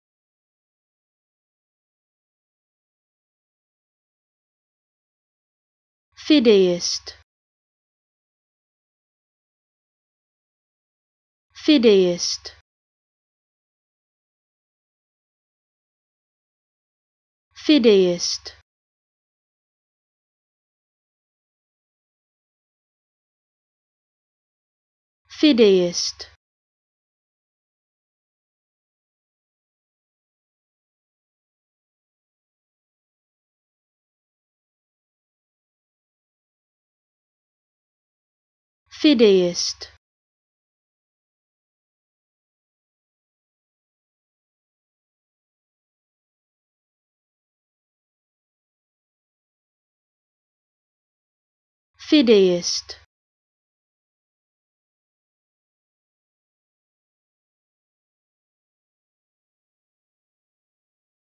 How To Say Fideist
Learn how to say Fideist with EmmaSaying free pronunciation tutorials.